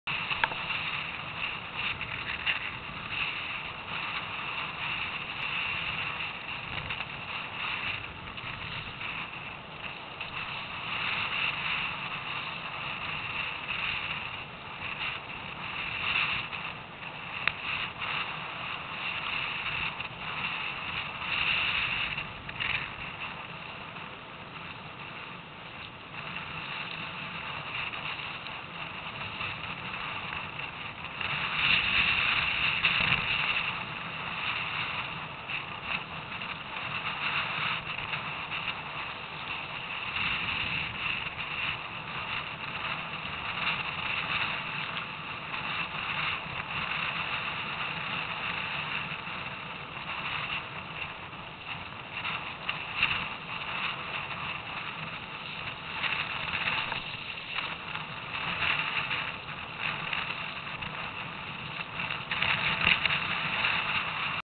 Rough waves, 20 knot headwind, on Narragansett Bay, RI
Sea kayaking on Narragansett Bay,  20 knot headwind, gusting to 25 knots, to Conimicut Point.  Summer 2008.